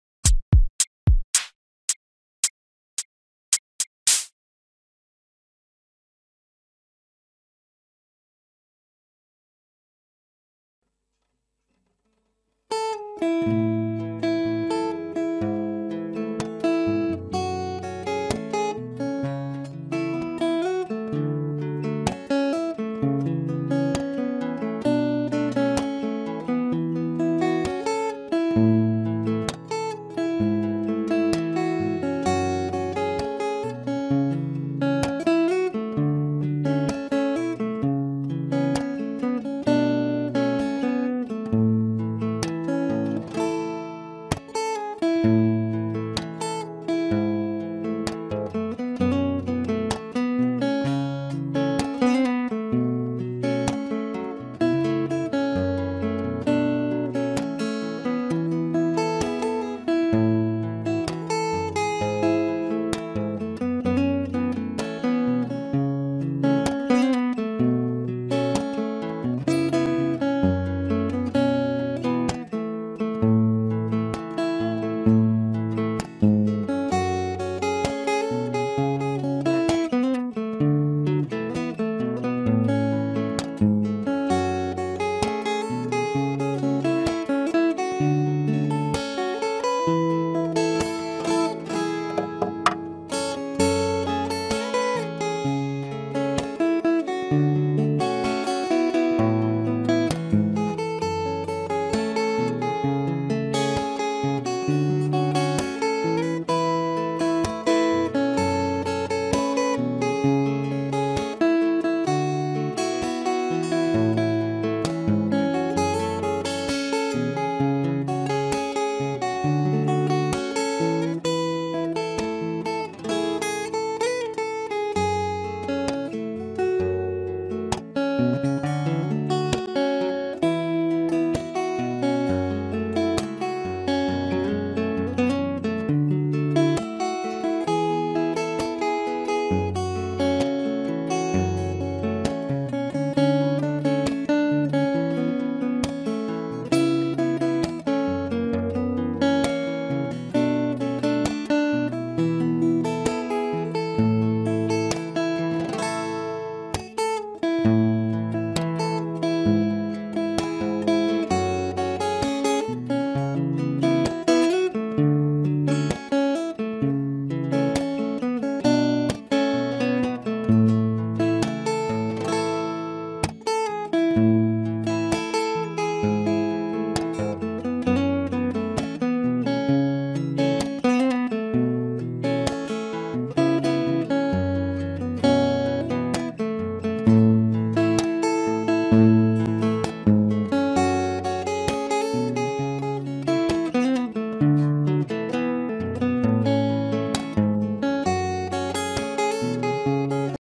Show Register button for timer killer | SSMMS tricks
Workspace guideline demo script Show register button for register timer killer. When register page loading many time, you can try Register button for submit before timer end. Make sure your disable Order auto submit.
Script nearly done, and will update for workspace on update site user as soon as possible.